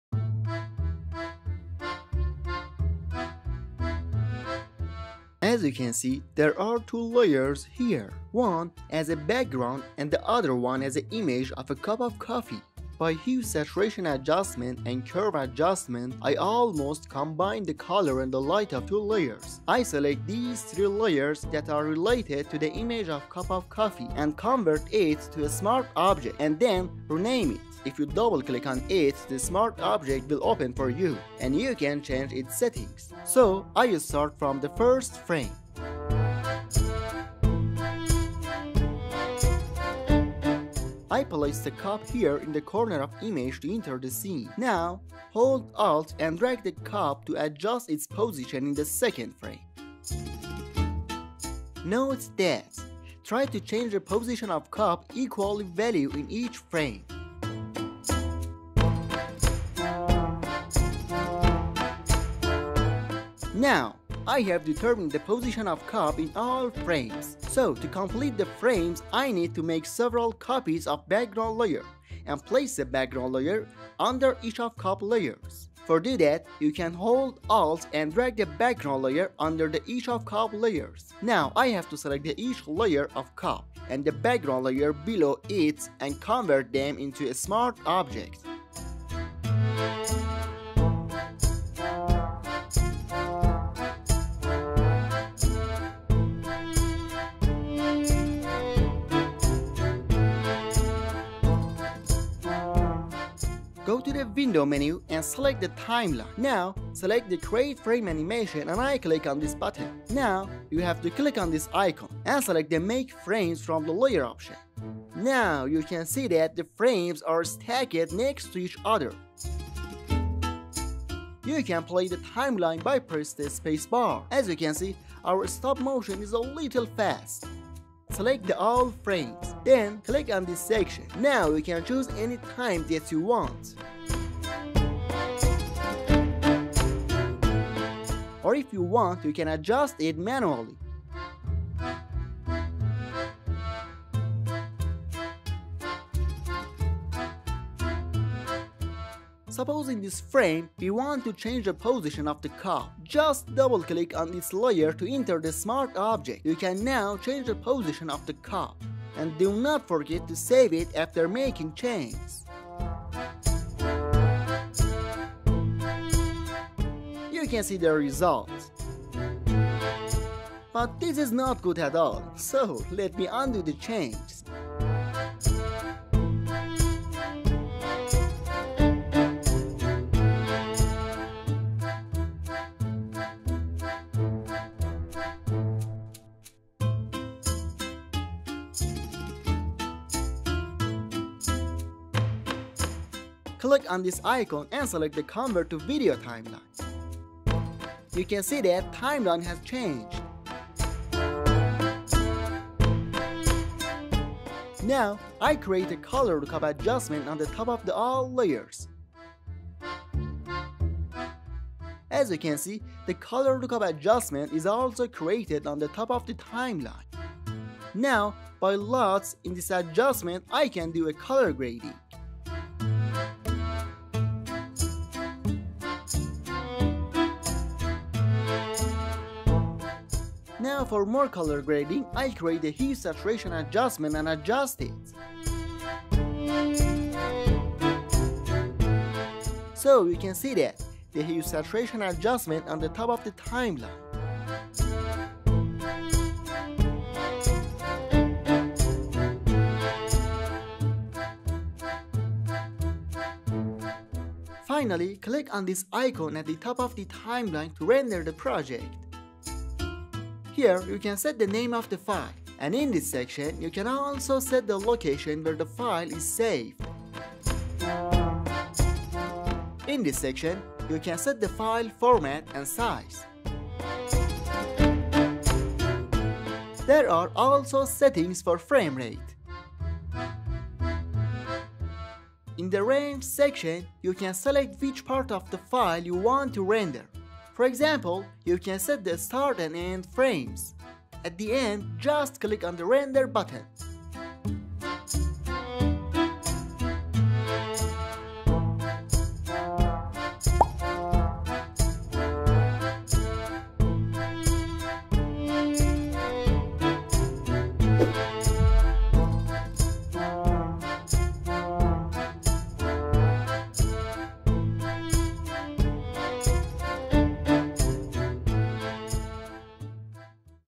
Stop Motion in Photoshop
In this tutorial you learn how to create stop motion in photoshop.

For better learning, we suggest you read more explanations of this tutorial on G-ART Tutorial

Subsisto motus effectus in Photoshop , फ़ोटोशॉप में स्टॉप मोशन प्रभाव , Photoshop中的定格動畫效果 , effet stop motion dans photoshop

Timecode
0:00 Intro
0:37 Make Frames
1:40 Import Frames to Timeline
2:04 Adjust the Speed
2:26 Edit Frames
3:10 Color Grading
4:11 Rendering